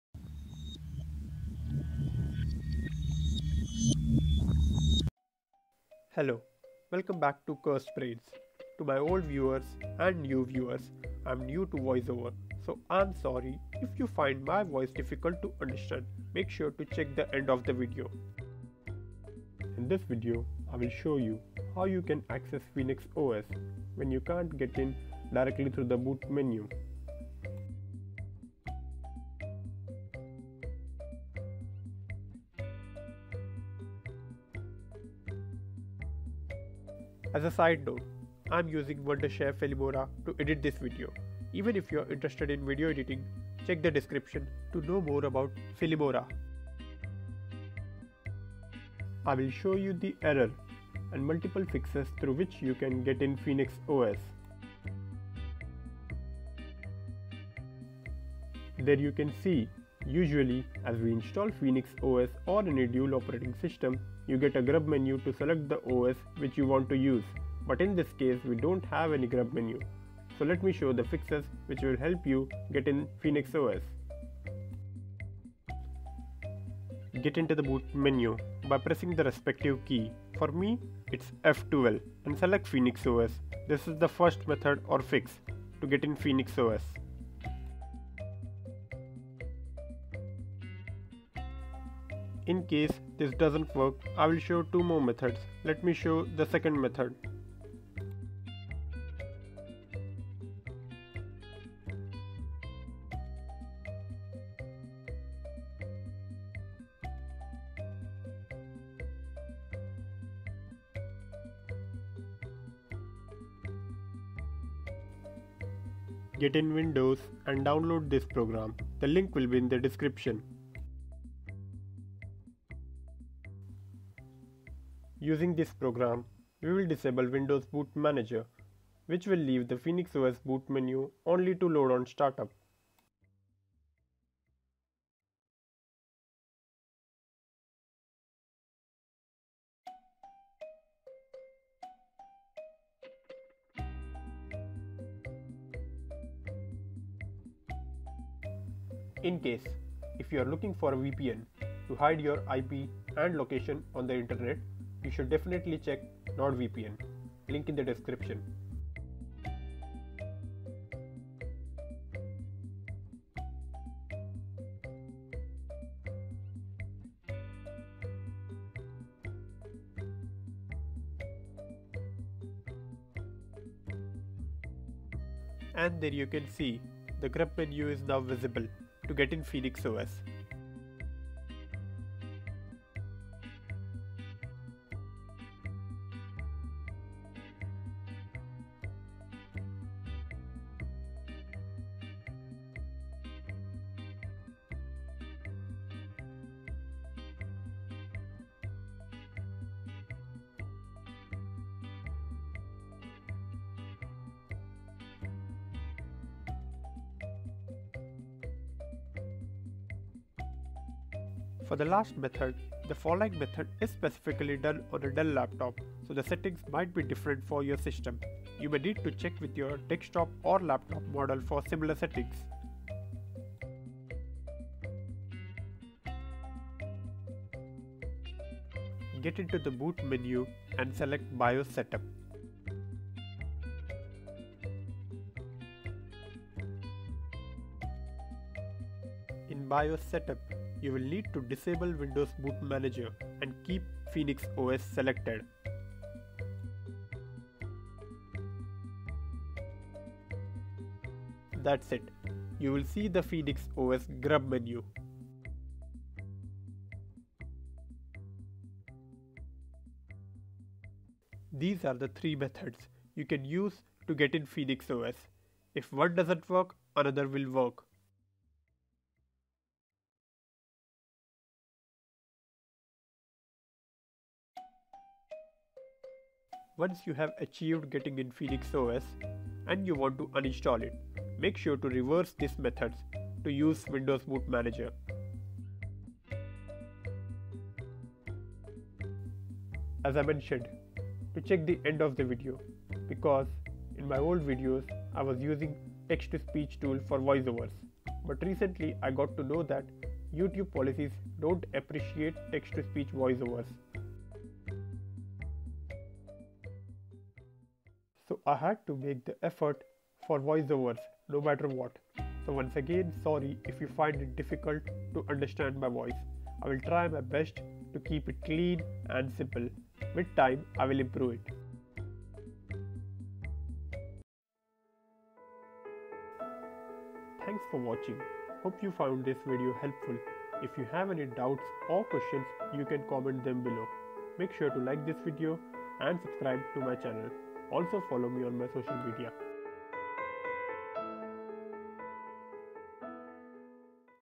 How to Enable Phoenix OS Boot Menu | Not Booting | Boot Problem Fixed
In this video, I will show you, how you can get in Phoenix OS when you can't access directly through the boot menu, how to show the boot menu on the startup of the system,

Phoenix OS is an Android OS which one can be dual boot with the primary operating system, Phoenix OS is used to install and use Android apps on a desktop,

for the past couple of years Pubg, free fire, and other battleground games is been famous on Android and many people want to play these games on a desktop, and Phoenix OS is been the most optimized and fast Android OS around,

but installing an Android OS such as Phoenix OS is been a little tricky which I already covered on How to Install Phoenix OS,

but even after installing many users have and still do face problems such as the found at /dev/sda or Phoenix OS won't boot at all and users don't get any boot menu to select the option while booting,

I showed the error & multiple fixes through which you can see the boot menu and get in Phoenix OS,

usually, as we install phoenix OS or any dual operating system, you get a grub menu to select the OS which you want to use, but in this case, we don't have any grub menu,

First Method -

Get into the boot menu by pressing the respective key for me its f12 & select phoenix OS this is the first method or fix to get in Phoenix OS.

Second Method -

using this program we will disable windows boot manager which will leave the Phoenix OS boot menu, only to load on startup, the grub menu will be visible to get in phoenix OS,

Third Method -

The following method is specifically done on a dell laptop, so the settings might be different for your system, you may need to check with your desktop or laptop model for similar settings,

get into the boot menu & select bios setup,

in bios setup, you will need to disable windows boot manager & keep phoenix OS selected, that's it you will see the phoenix OS grub menu,

these are three methods, you can use to fix the boot menu not appearing and get in Phoenix OS, if one doesn't work, another will work,

once you have achieved getting in phoenix OS, & you want to uninstall it, make sure to reverse these methods, to use Windows boot manager to get in Windows then you can watch my video on How to Uninstall Phoenix OS easily,

Time Stamps -
0:00 Intro
0:50 Phoenix OS Boot Error
1:13 First Method Select Phoenix OS from Boot Menu
01:48 Second Method Disable Windows Boot Manager using Bootice
3:47 Third Method Disable Windows Boot Manager through Bios Setup

Leave your comments & doubts in the comment section,

Social -